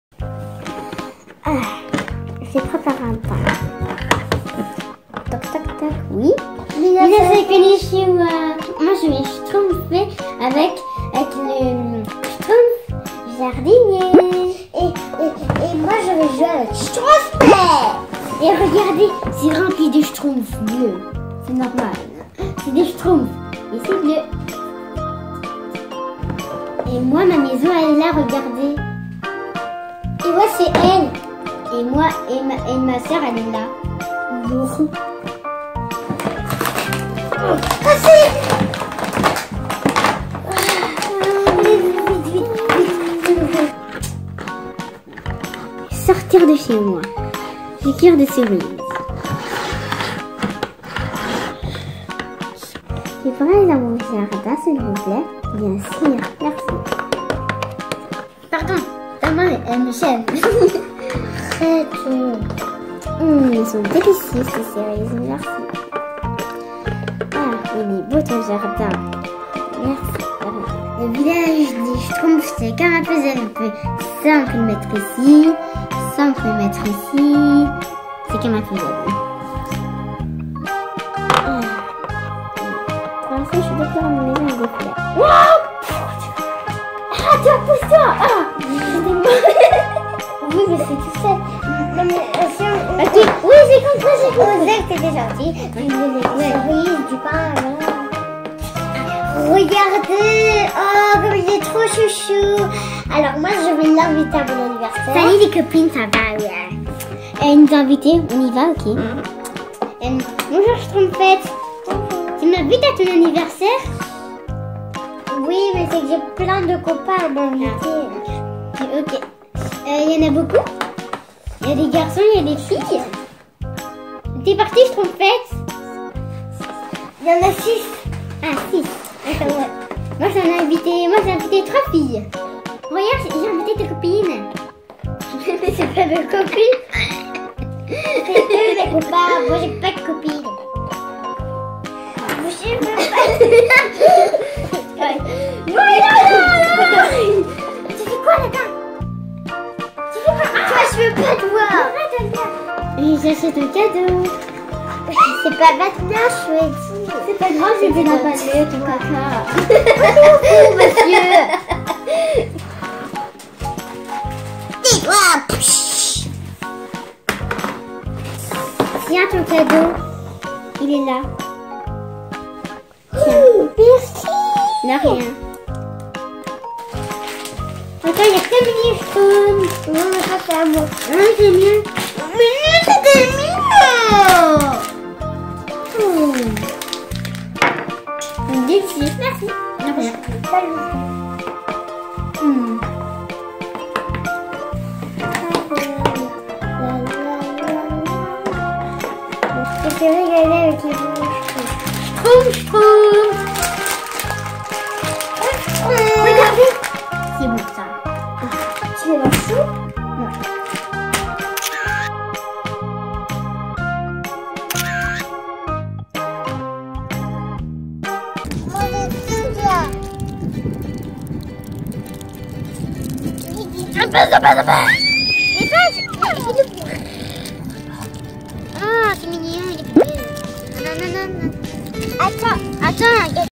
Découverte du monde des Schtroumpfs Haribo - Discover the world of the Smurfs - Unboxing
★Assya : Nous allons vous Schtroumpfer ces petits  êtres bleus, drôles, mignons, petits et surtout très rusé. D'un côté nous avons Schtrumpf cuisinier et de l'autre, la belle et ravissante Schtrumpfette.

Je trouve que les bonbons Haribo Schtroumps ont meilleurs goûts :)★